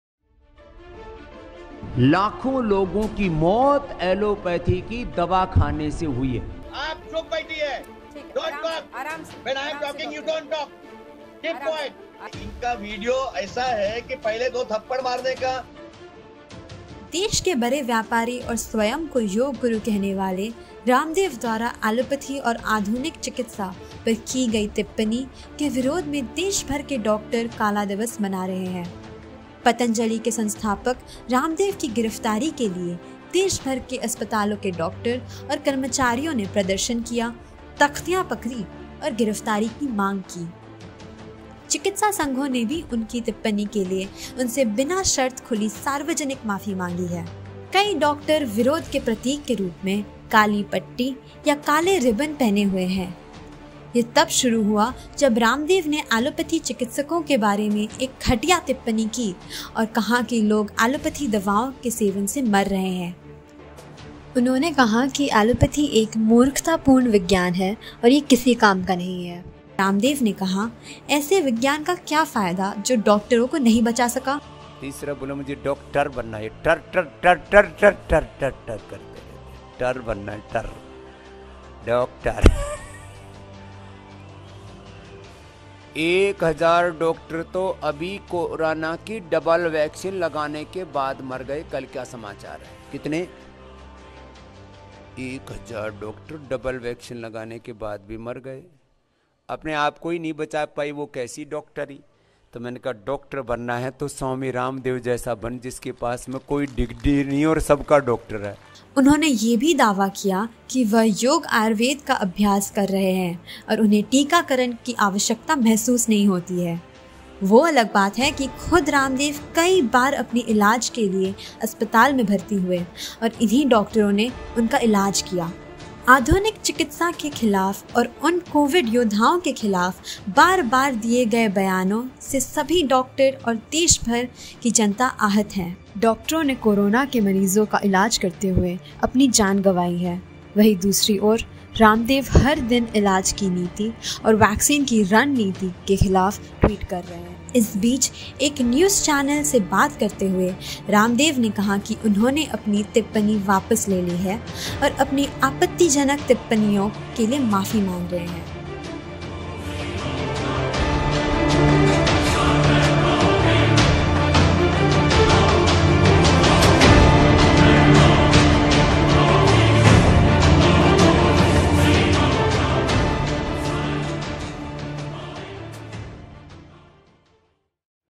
Ramdev's Comments on Allopathy एलोपैथी पर रामदेव विचार
This video is on the irresponsible comments made by Ramdev against allopathy practitioners. In response doctors and medical staff all over the country protested against him. The reason for making this video is to inform people that Ramdev has extremely problematic views on most subjects and everything he says shouldn't be viewed as the ultimate truth.

ये वीडियो रामदेव द्वारा एलोपैथी चिकित्सकों के ख़िलाफ़ दिए गए ग़ैर ज़िम्मेदाराना बयान को लेकर बनाया गया है। इस बयान के ख़िलाफ़ देश भर के  चिकित्सकों और कर्मचारियों ने प्रदर्शन किये। इस वीडियो  को बनाने की सबसे बड़ी वजह है रामदेव के अजीबो-ग़रीब विचारों को लोगों के साथ साझा करना और साथ ही ये बताना कि रामदेव द्वारा कही हर बात को  परम सत्य नहीं मानना चाहिए।

Research and Script - Suvam
Editing - Shivam
Voice Over - Ananya